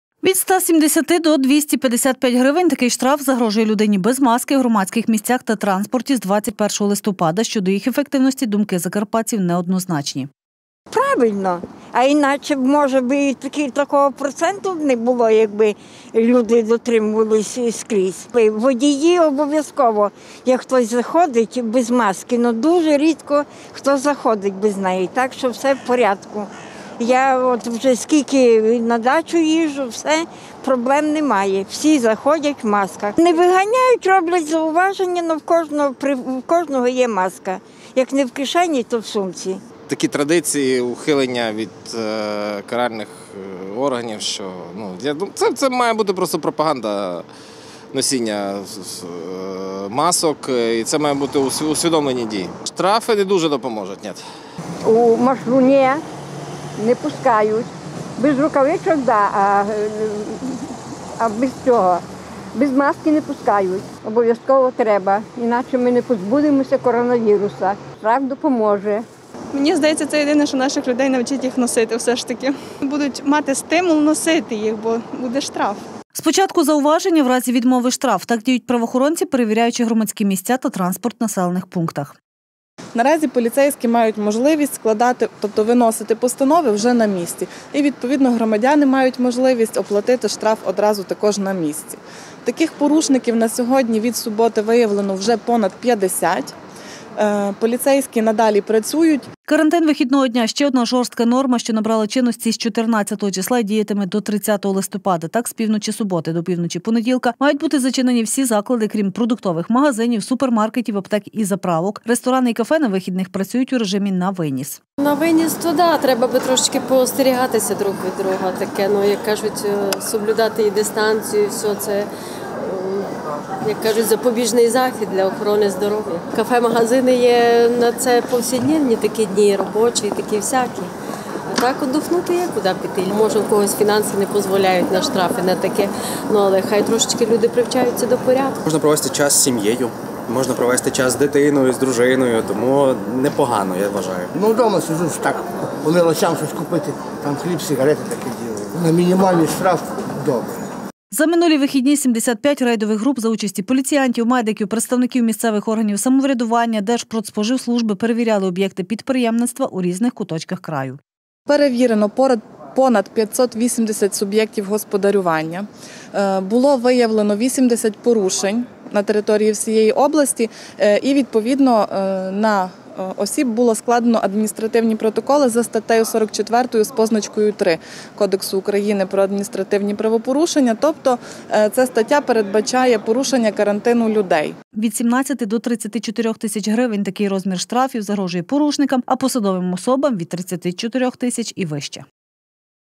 Покарання на місці. Штафують за порушення карантину
Від 170 до 255 грн – такий штраф загрожує людині без маски у громадських місцях та транспорті з  21 листопада 2020 року. Щодо їх ефективності думки закараптців неоднозначні.  Переважна  більшість про те, що лиш штрафами можна спонукати до носіння масок.  Спочатку зауваження, а у разі відмови – штраф – так діють правоохоронці, перевіряючи громадські місця та транспорт у населених пунктах Закарпаття. Коментар  Ганна Дан, речниця ГУ НПУ в Закарпатській області .  Карантин вихідного дня -ще одна жорстка норма, що набрала чинності з 14 листопада і діятиме до 30. Так, з півночі суботи до півночі понеділка мають бути зачинені всі  заклади, крім продуктових магазинів , супермаркетів, аптек і заправок. Ресторани і кафе на вихідних працюють у режимі «на виніс».За минулі вихідні 75 рейдових груп за участі поліціянтів, медиків, представників місцевих органів самоврядування, працівників держпродспоживслужби  перевіряли суб’єкти підприємництва у різних куточках краю. Від 17 до 34 тисяч гривень і для  – такий розмір штрафів загрожує порушникам, а посадовим особам від 34 тисяч гривень і вище.